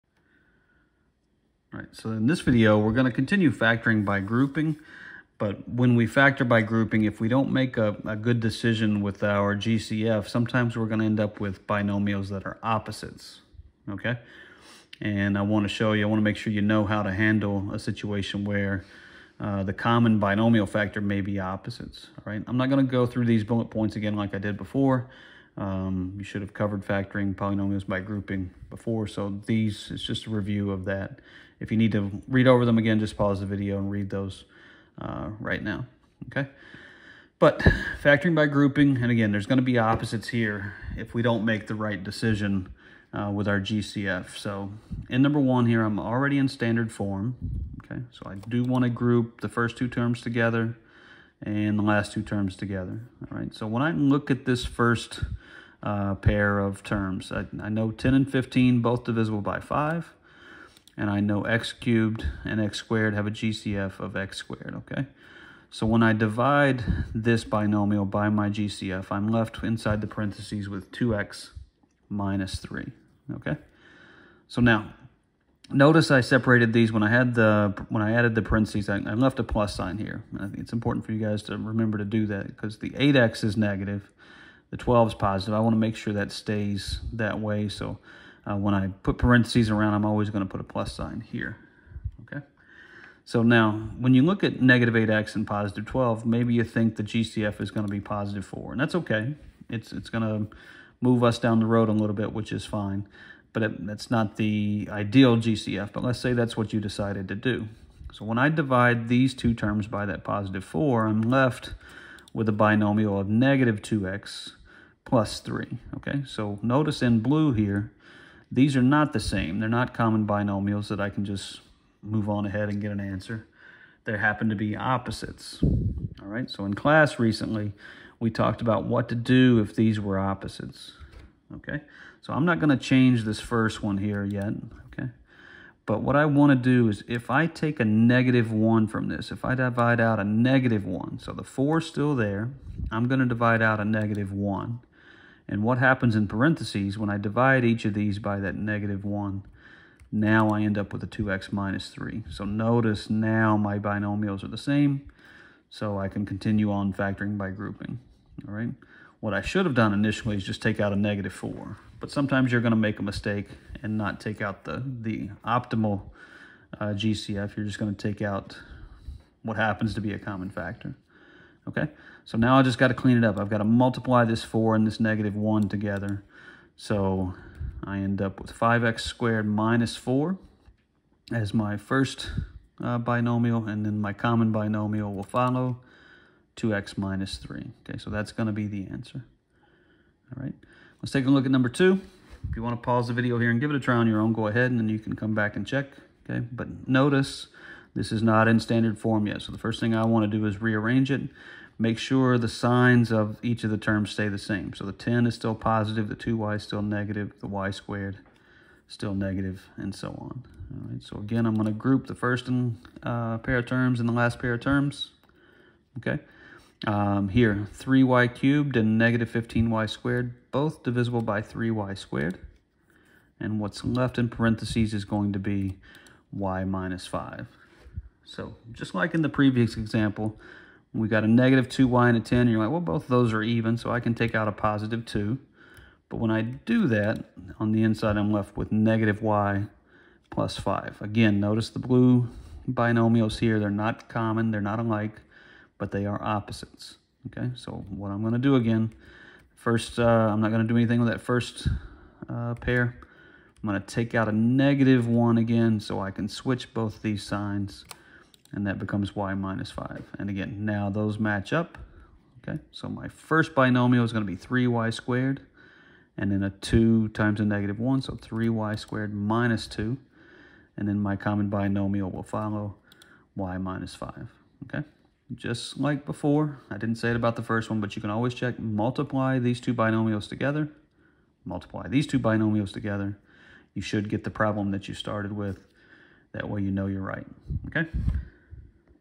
Factoring Polynomials by Grouping - with Opposites
In this video, we work two examples where the common binomials don’t match initially…they’re opposites. We go over what to do in that case.